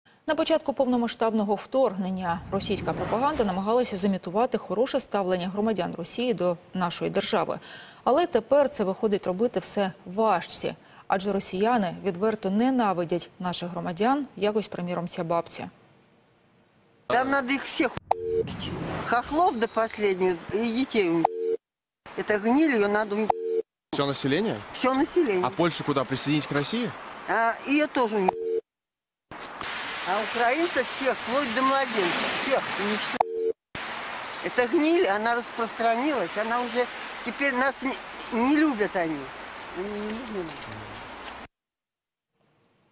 "Они нас не любят" - бабця з Росії закликала винищити українську та польську націю
Російська пенсіонерка закликає знищити всіх українців.